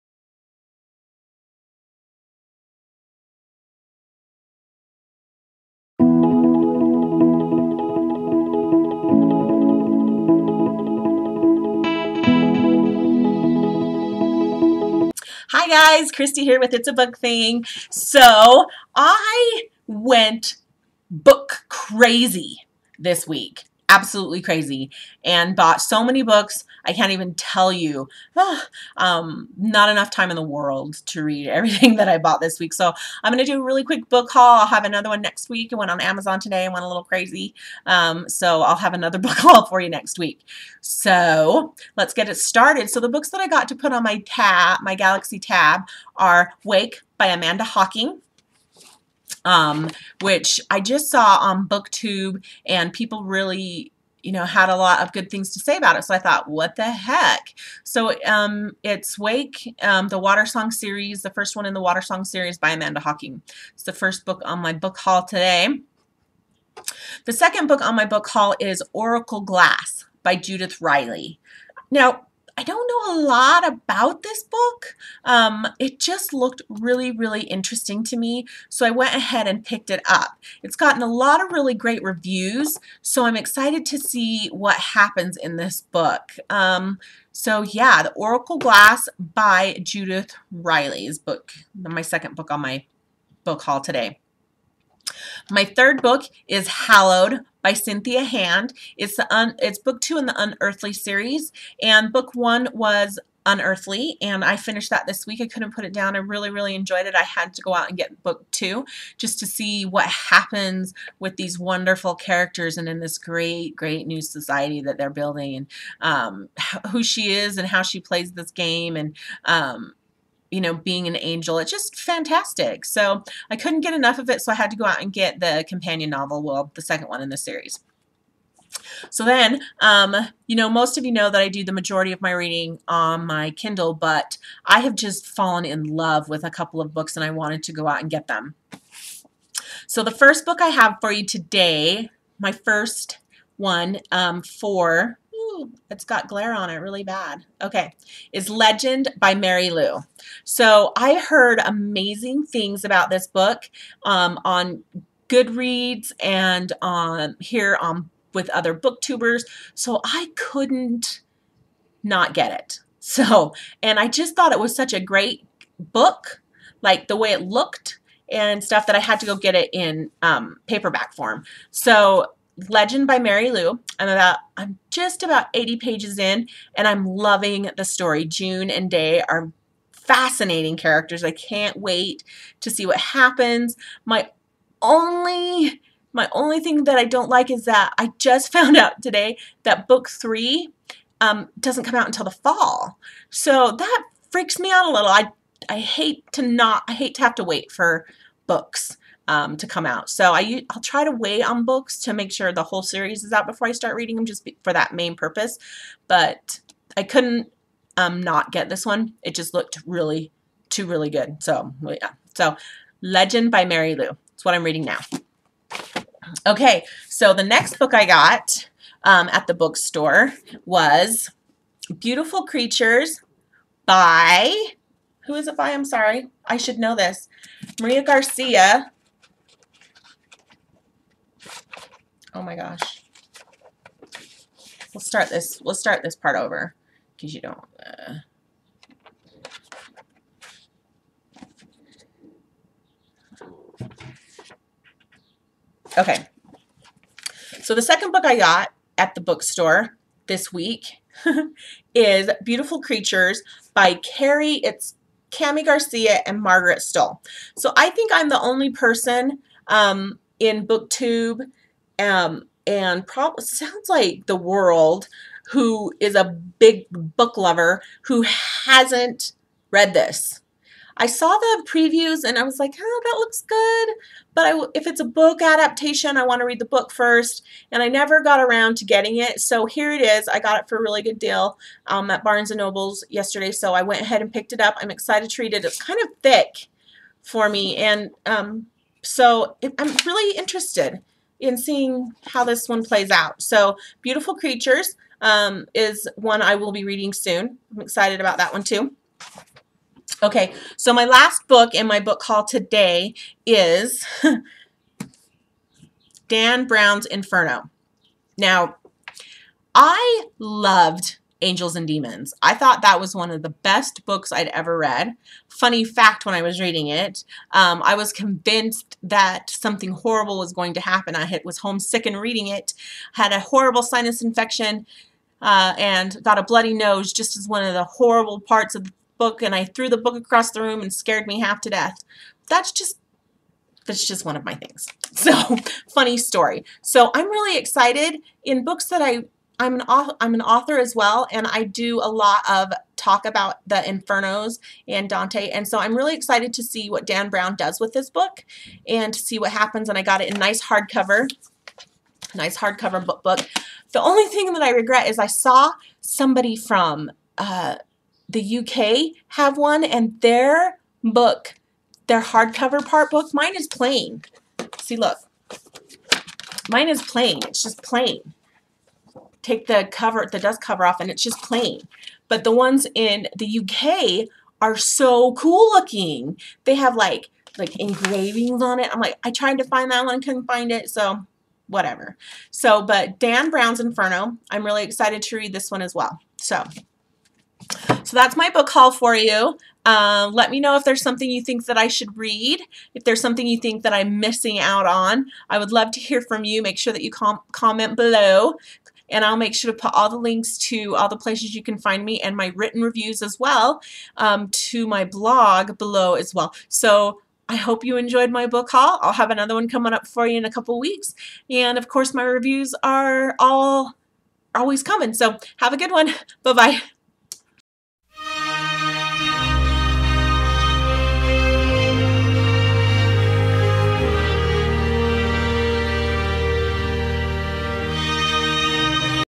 Book Haul 0624213
Another great book haul. I think I might have a book buying problem...wait I know I have a book buying problem.

Author -- Urban Fantasy Romance , book reviewer, blogger, vlogger
Where Fantasy Meets Reality